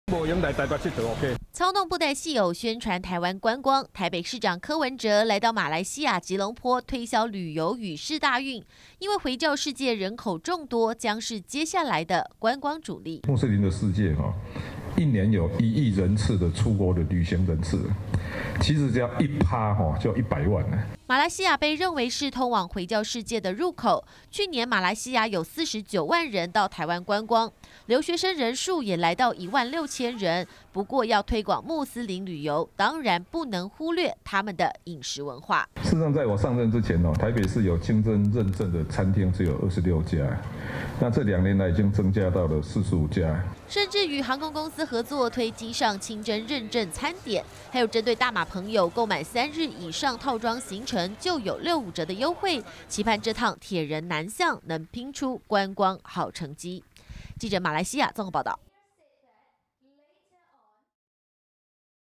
20170328中天新聞 柯P南向拚觀光 搶穆斯林旅遊商機
台北市長柯文哲率團前往馬來西亞推銷旅遊與世大運，甚至與航空公司聯手，推機上清真認證餐點，因為回教世界一年有，高達一億人出國旅遊，將是接下來的觀光主力。